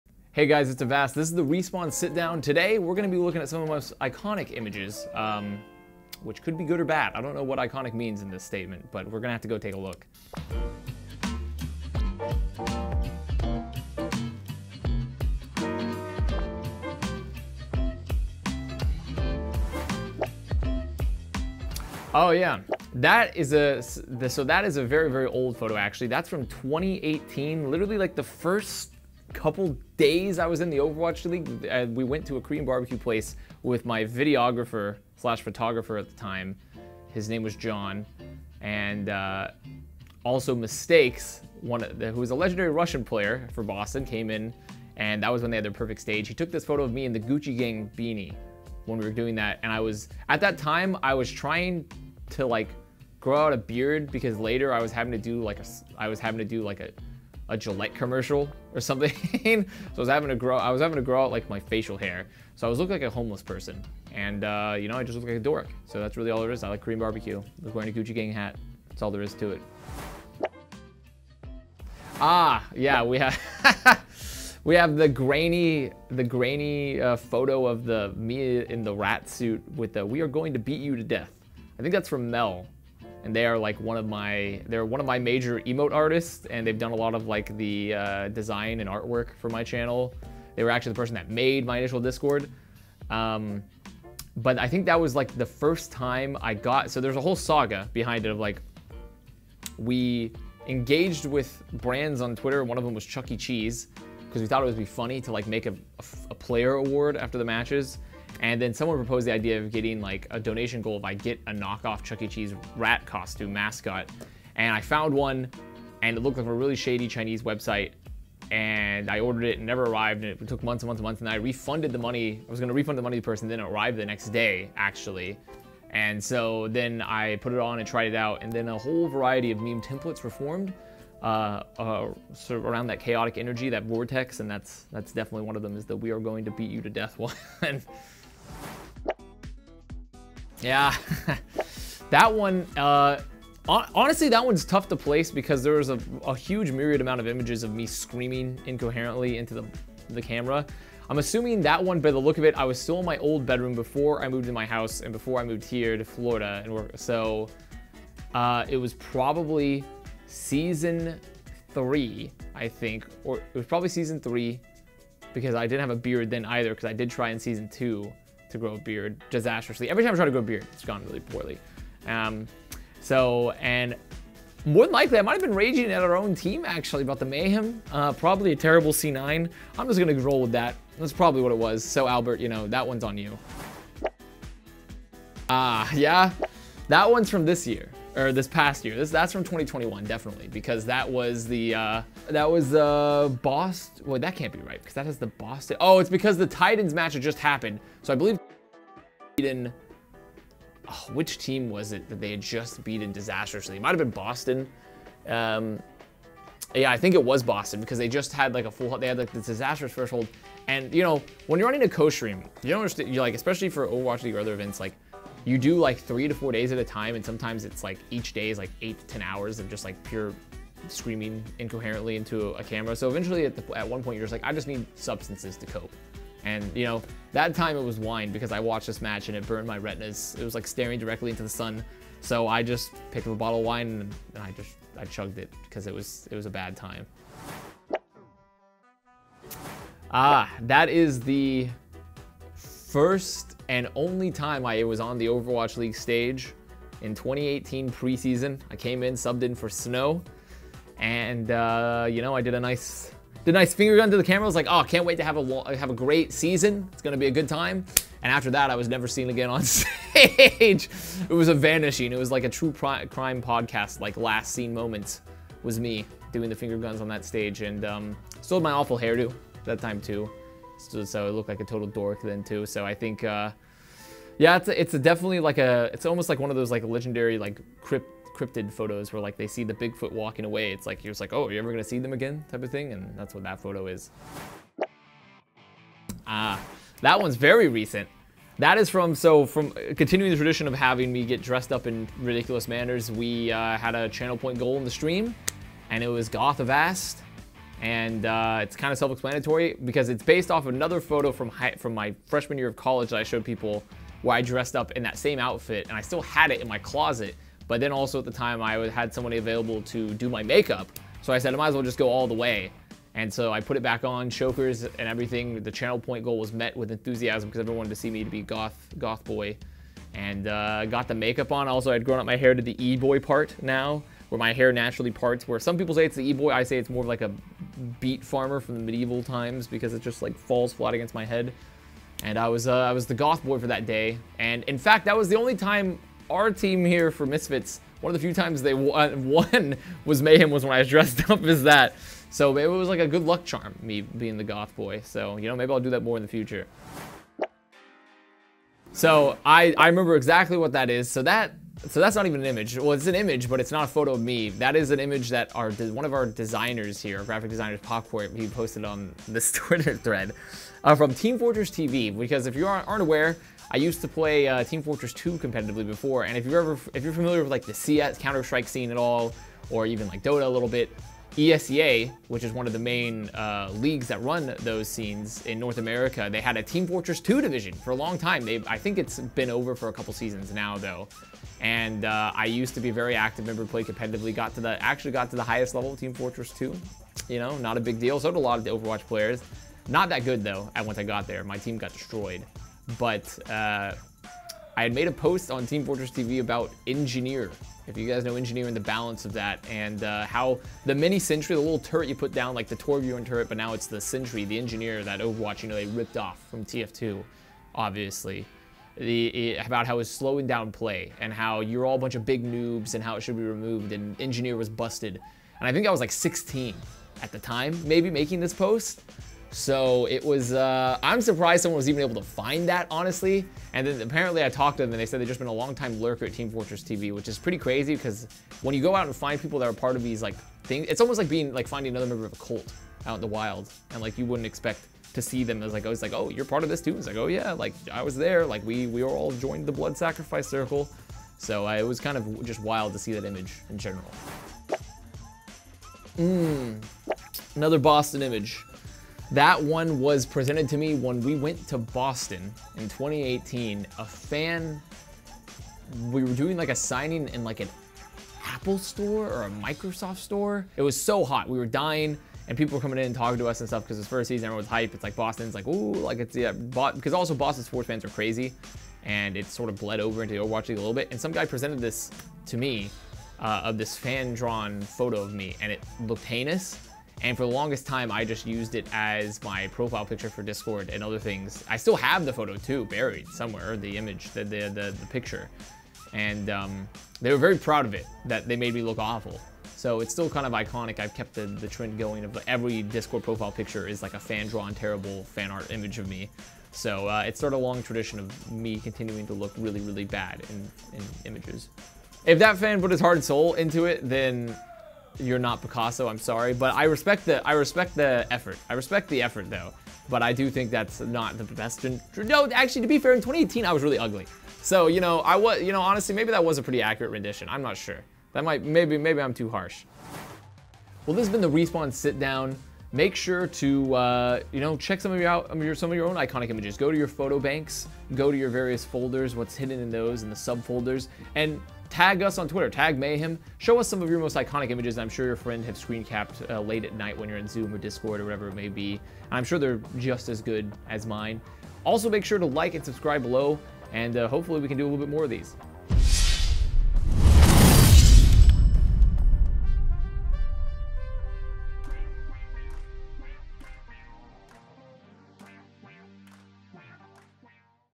Avast Looks at His Most Iconic Images | Respawn Sitdown
Presented by Respawn. Avast reflects on some of the most iconic images attached to his name. Including images of Chuck E Cheese, Bren, and his short lived Boston OWL career in the 2018 preseason.